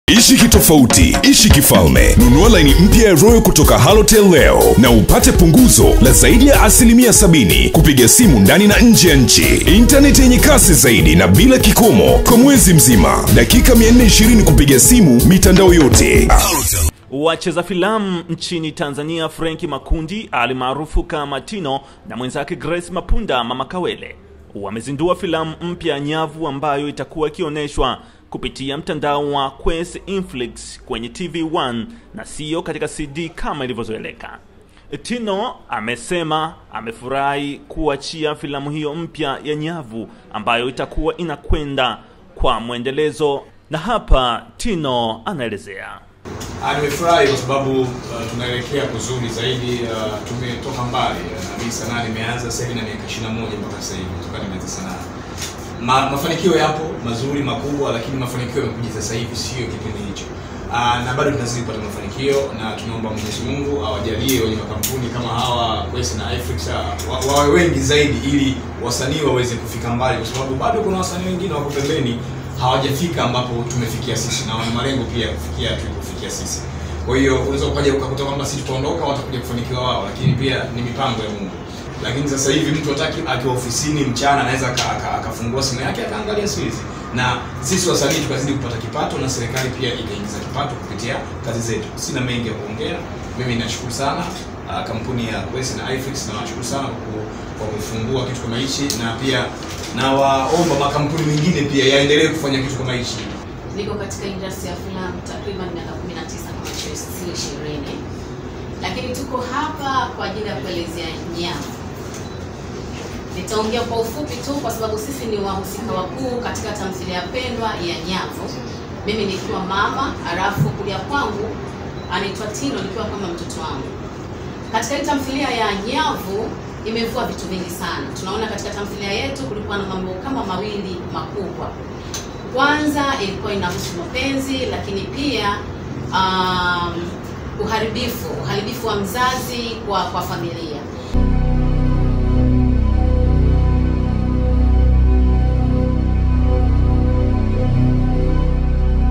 MWIGIZAJI TINO AJA NA MBINU MPYA KUUZA FILAMU
WACHEZA filamu Tanzania, Frank Makundi, maarufu 'Tino'
na mwenzake Grace Mapunda 'Mama Kawele' wamezindua filamu mpya ya Nyavu ambayo itakuwa ikioneshwa kupitia mtandao wa Kwese Inflix kwenye TV 1 na siyo katika CD, kama ilivyozoeleka.